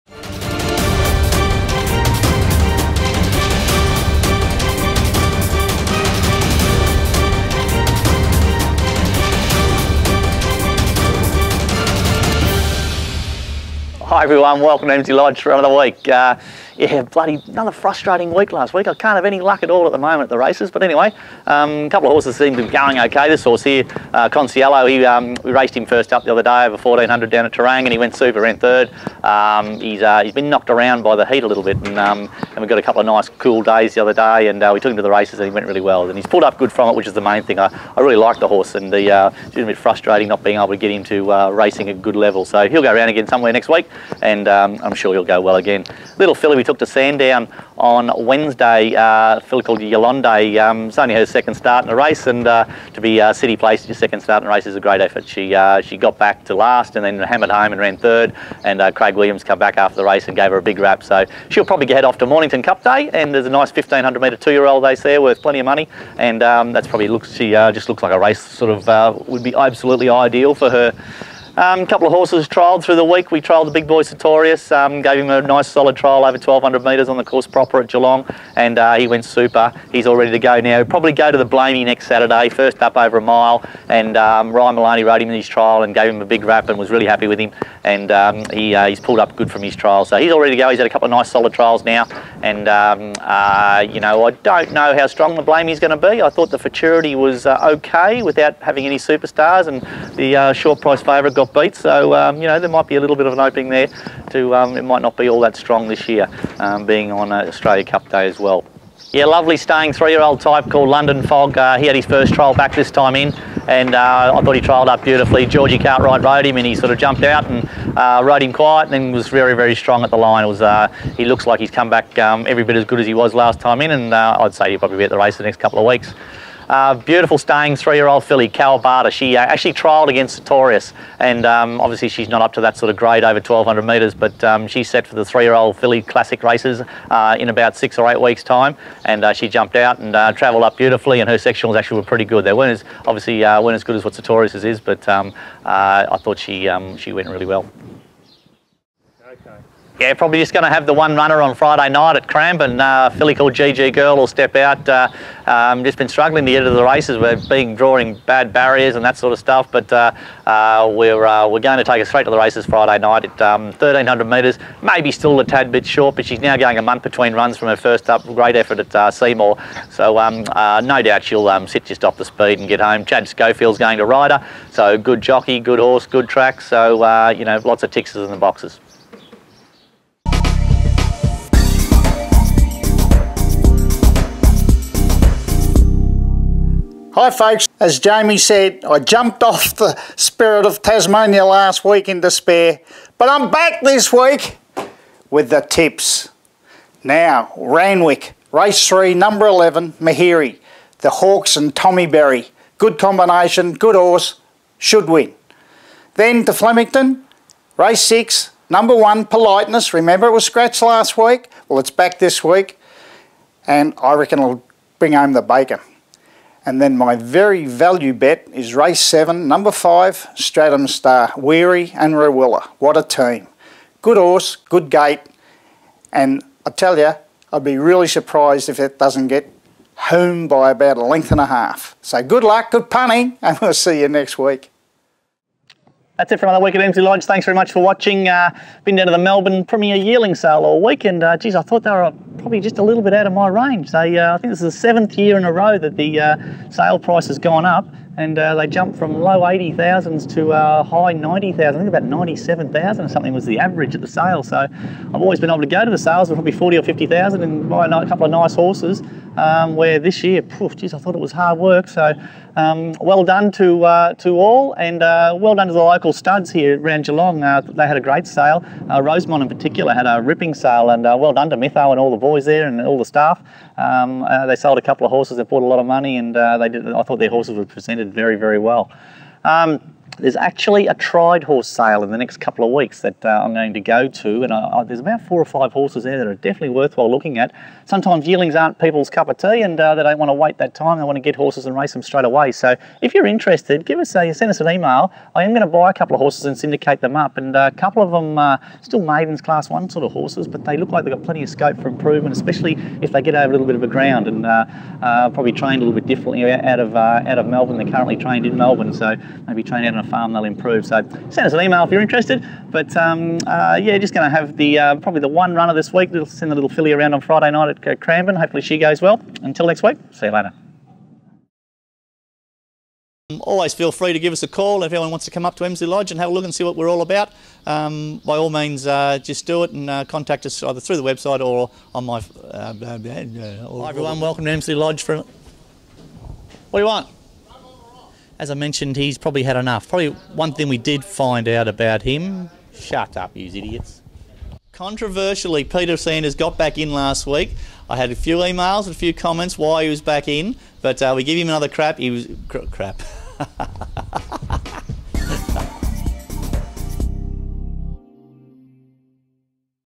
March 7 2015 - Emsley Lodge Report
AN interesting week with two placegetters at huge odds while Sertorius showed he was nearing his top after an impressive Geelong trial against stablemate Kawabata. London Fog also showed out in a barrier trial while we capture Gigi Girl in full flight in a track gallop ahead of this week's assignment. Jamie also spent a week at the yearling sales and the stable has plans for more purchases in the coming weeks. Peter Sandars makes a surprise return with his tips for the week and the bloopers are sure get a laugh.